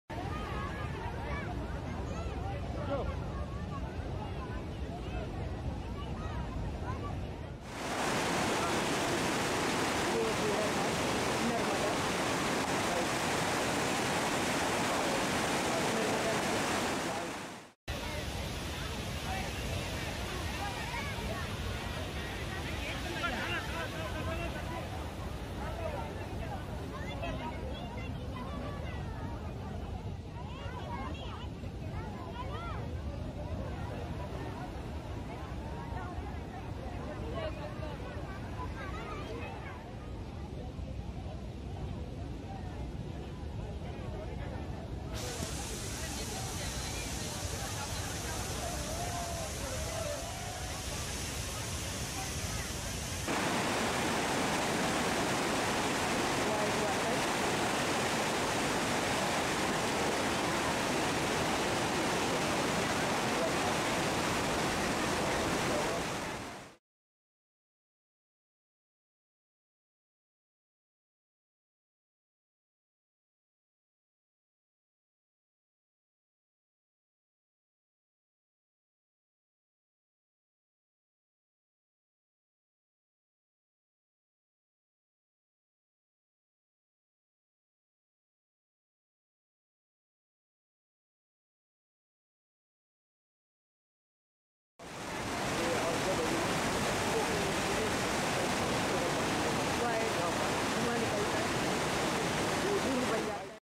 Narmada River Dam amazing Water Fall
this is naramada holy river of Madhaypradesh, it is very amazing nature beauty, This documentary is by Bollywood Jhugi Star..